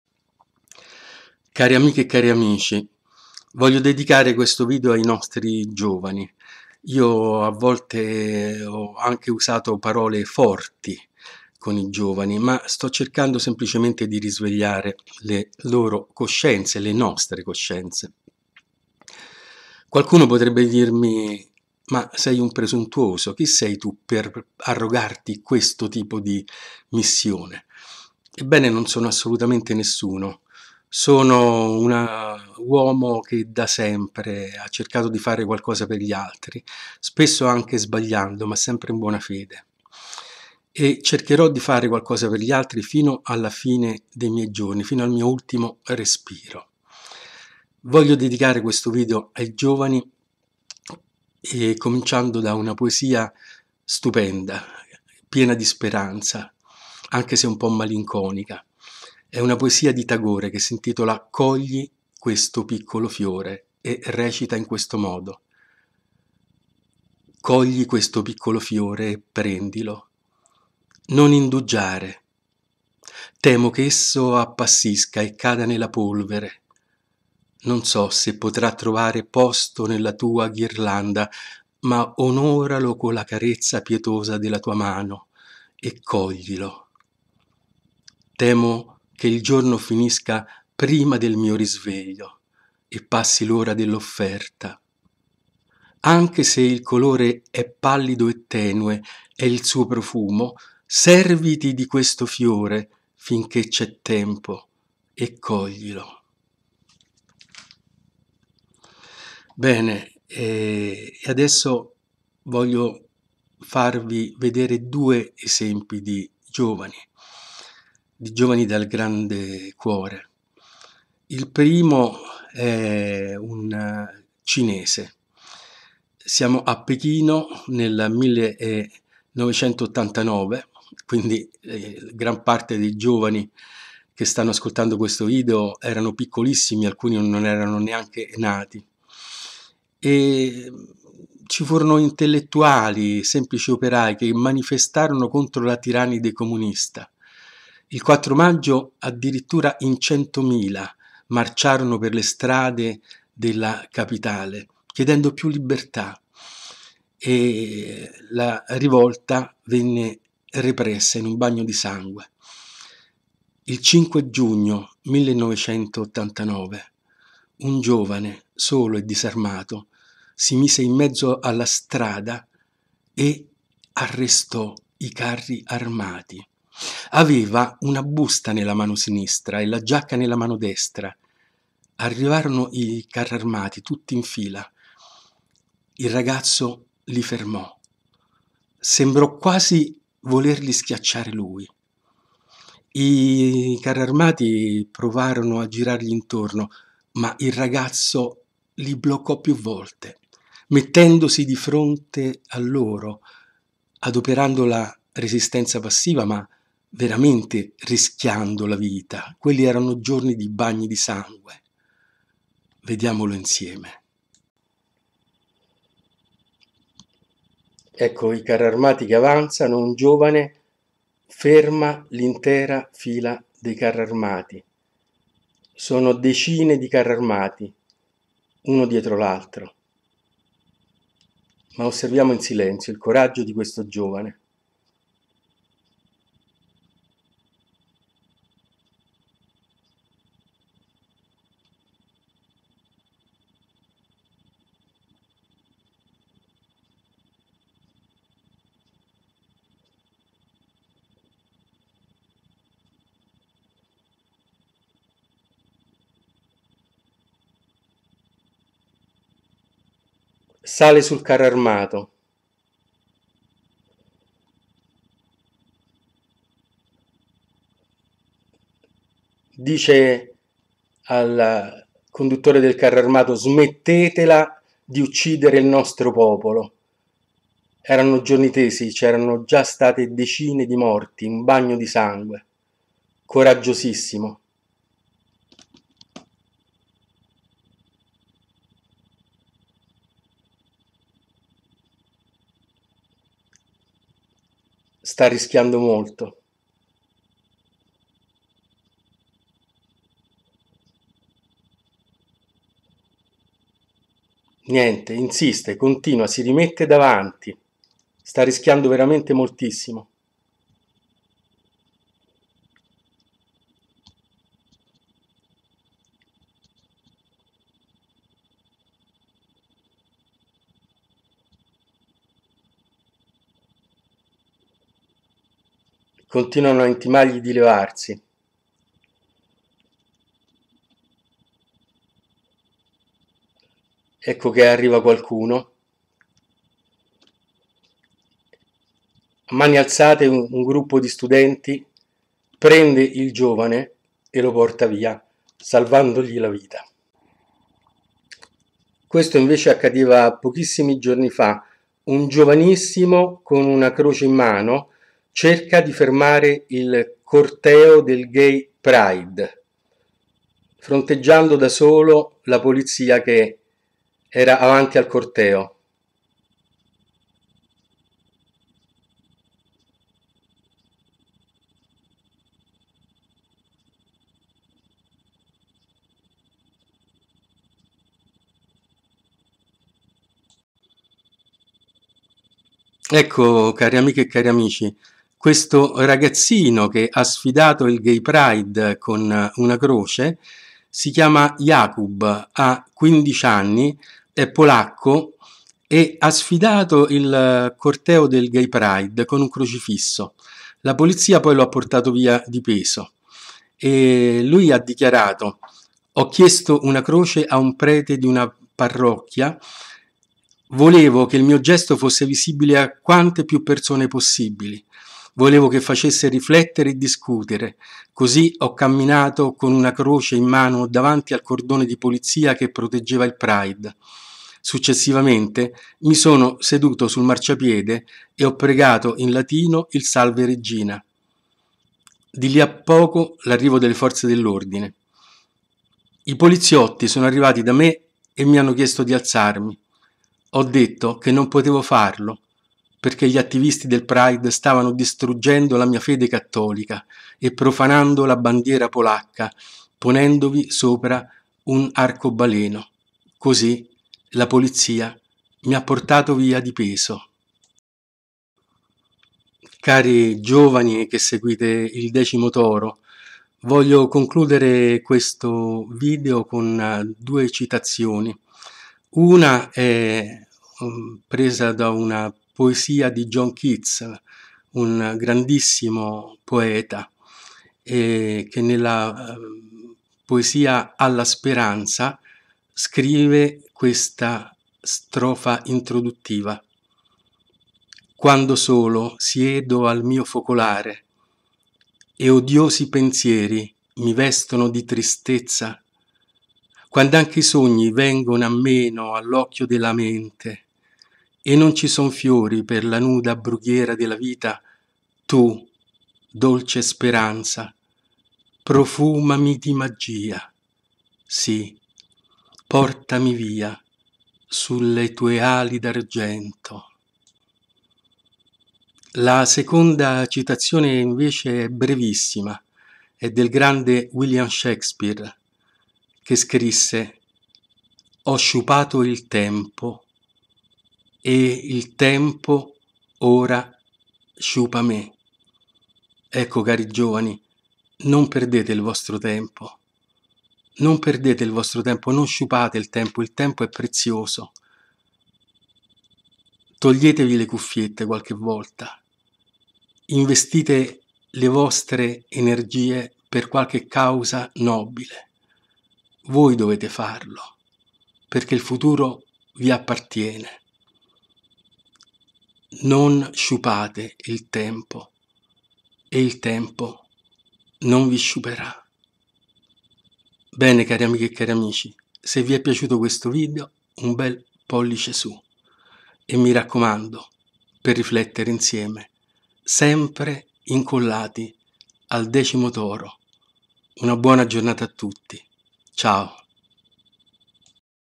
Giovani, venite, lottiamo insieme!
Un invito del Decimo Toro ai giovani:
"Levatevi quelle maledette cuffiette e tornate alla realtà. E' tempo di lotta!"